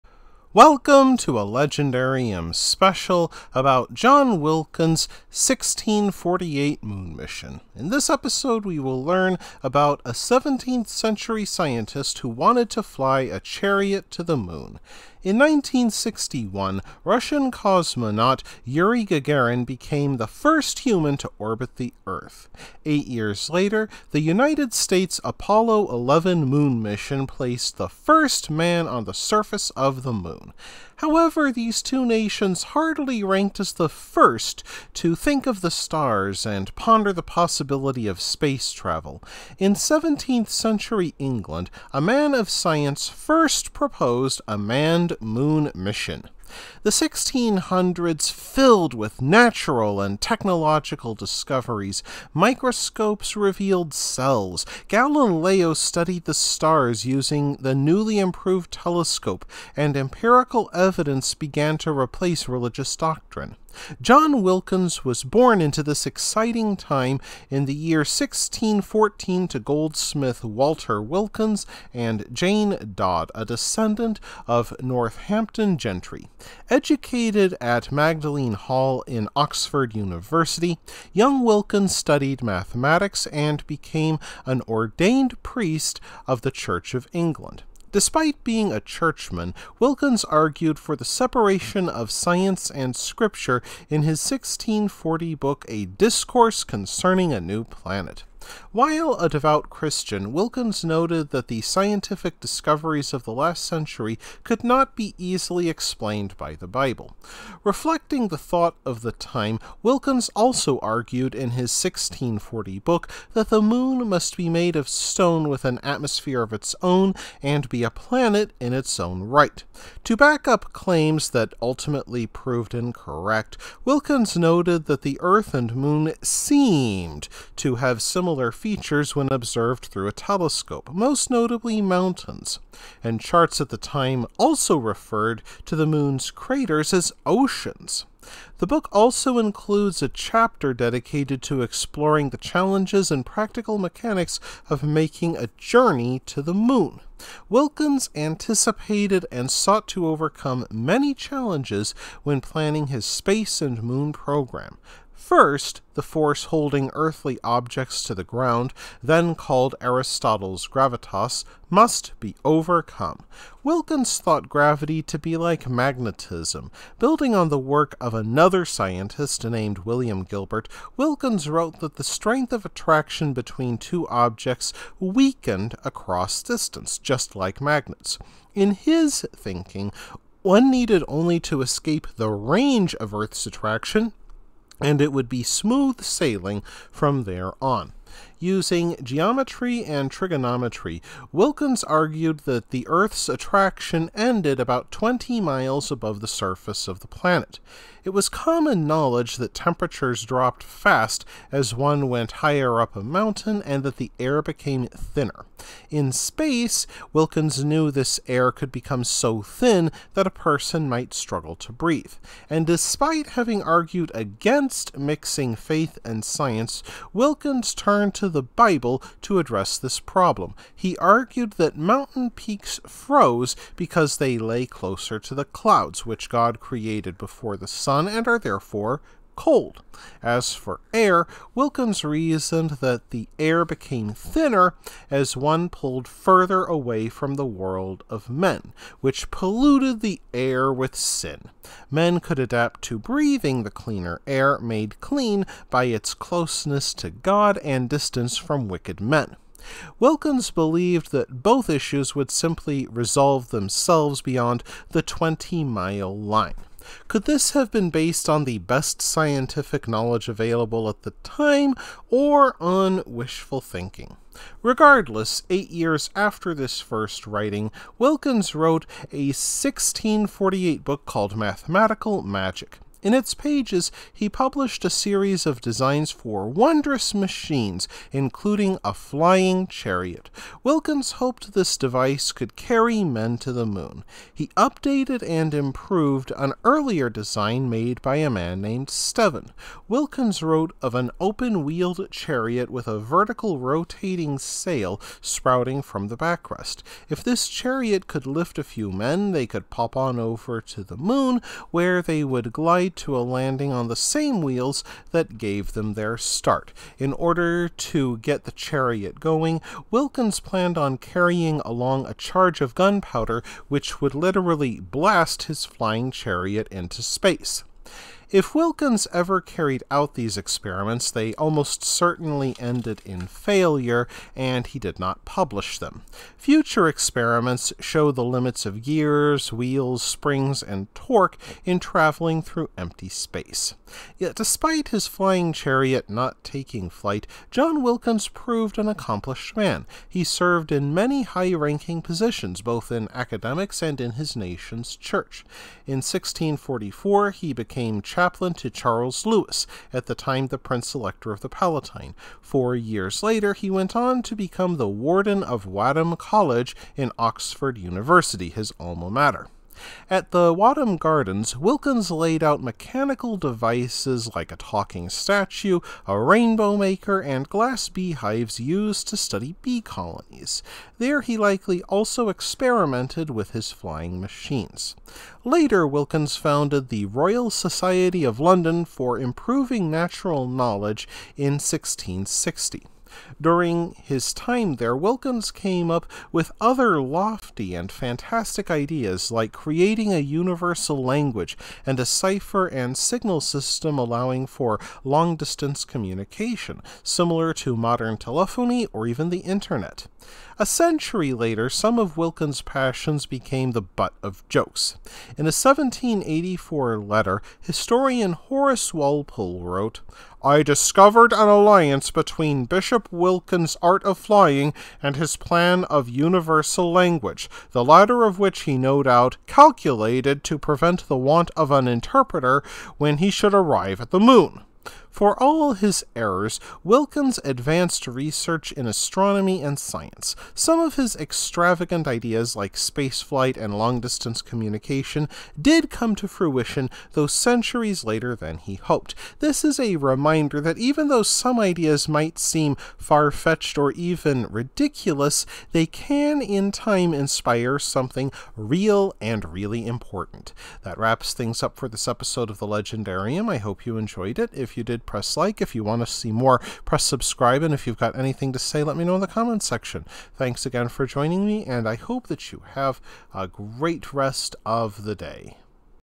John Wilkins' 1648 Moon Mission
Most of us know about the first Moon landing in 1969. Yet, few of us know that more than 350 years before Apollo 11 took flight, an English scientist named John Wilkins sketched out an idea for flying to the Moon. It involved a flying chariot, gunpowder, and a lot of wishful thinking.